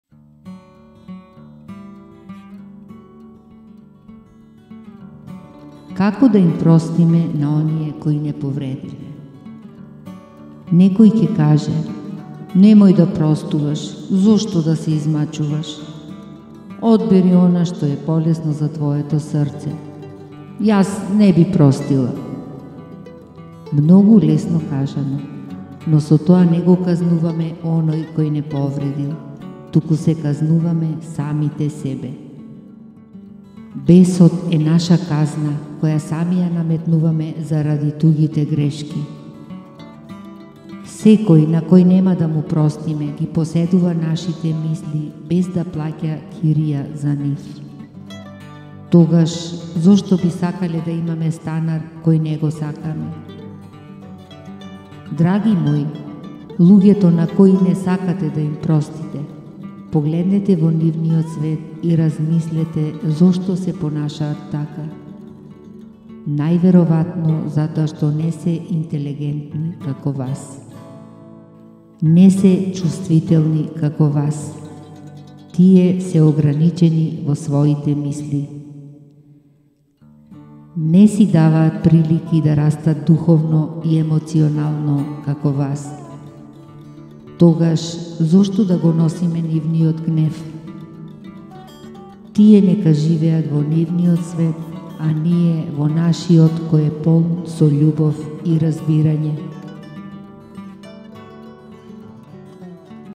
Stanarot koj ne plaḱa ḱirija!!!
КАКО ДА ИМ ПРОСТИМЕ НА ОНИЕ КОИ НЕ ПОВРЕДИЛЕ!!!
НЕКОЈ ЌЕ КАЖЕ - НЕМОЈ ДА ПРОСТУВАШ, ЗОШТО ДА СЕ ИЗМАЧУВАШ.
ОДБЕРИ ОНА ШТО Е ПОЛЕСНО ЗА ТВОЕТО СРЦЕ.
НЕ ПРОСТУВАЈ.
МНОГУ ЛЕСНО КАЖАНО, НО СО ТОА НЕ ГО КАЗНУВАМЕ ОНОЈ КОЈ НЕ ПОВРЕДИЛ, ТУКУ СЕ  КАЗНУВАМЕ САМИТЕ СЕБЕ.
БЕСОТ Е НАША КАЗНА, КОЈА САМИ ЈА НАМЕТНУВАМЕ ЗАРАДИ ТУЃИТЕ ГРЕШКИ.
СЕКОЈ НА КОЈ НЕМА ДА МУ ПРОСТИМЕ ГИ ПОСЕДУВА НАШИТЕ МИСЛИ БЕЗ ДА ПЛАЌА КИРИЈА.
ТОГАШ ЗОШТО БИ САКАЛЕ ДА ИМАМЕ СТАНАР КОЈ НЕ ГО САКАМЕ.
ДРАГИ МОИ, ЛУЃЕТО НА КОИ НЕ САКАТЕ ДА ИМ ПРОСТИТЕ, ПОГЛЕДНЕТЕ ВО НИВНИОТ СВЕТ И РАЗМИСЛЕТЕ, ЗОШТО СЕ ПОНАШААТ ТАКА.
НАЈВЕРОВАТНО ЗАТОА ШТО НЕ СЕ ИНТЕЛЕГЕНТНИ КАКО ВАС.
НЕ СЕ ЧУВСТВИТЕЛНИКАКО ВАС, ТИЕ СЕ ОГРАНИЧЕНИ ВО СВОИТЕ МИСЛИ.
НЕ СИ ДАВААТ ПРИЛИКИ ДА РАСТАТ ДУХОВНО И ЕМОЦИОНАЛНО КАКО ВАС.
ТОГАШ ЗОШТО ДА ГО НОСИМЕ НИВНИОТ ГНЕВ.
ТИЕ НЕКА ЖИВЕАТ ВО НИВНИОТ СВЕТ, А НИЕ ВО НАШИОТ КОЈ Е ПОЛН СО ЉУБОВ И РАЗБИРАЊЕ.